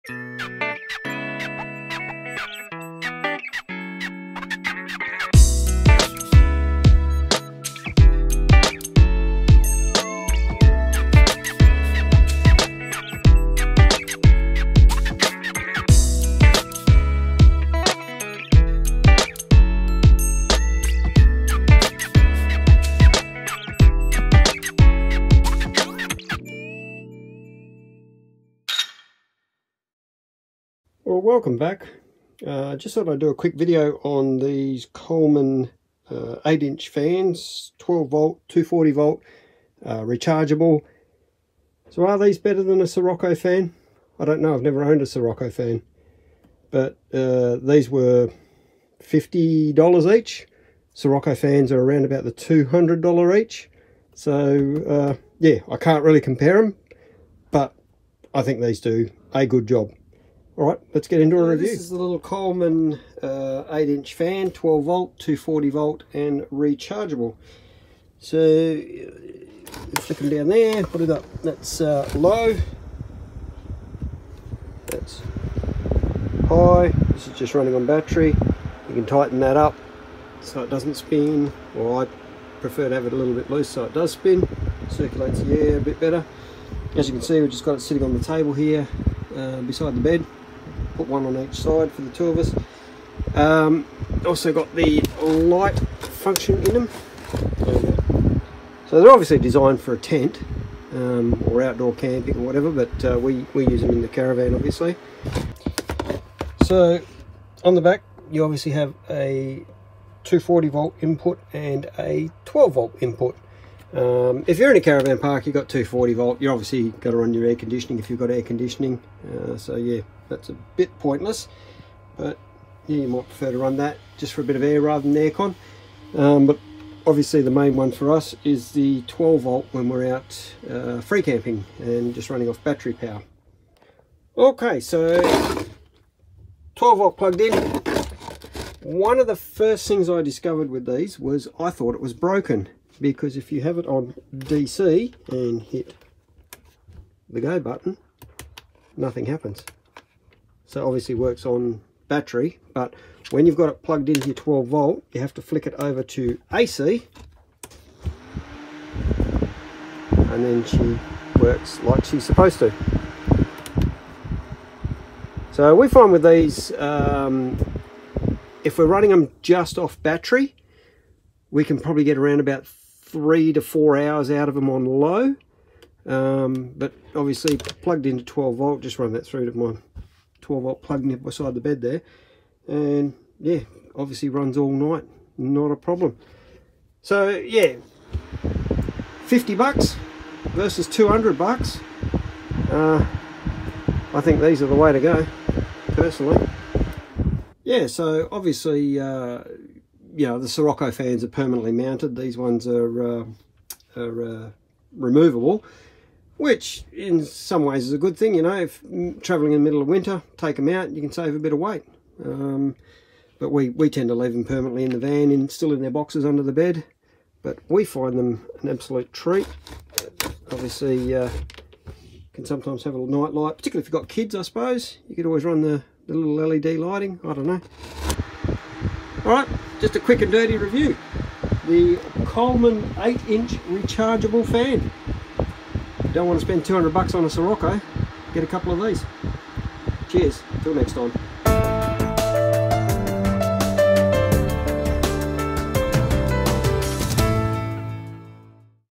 Coleman 8" Fan Review - Better than Sirocco Fans?
Not wanting to spend $200 each on Sirocco fans, we decided to give the Coleman 8" rechargeable fans a go at $50 each.

After almost 2 years, I'm happy with their performance and decided to share my review and thoughts on them.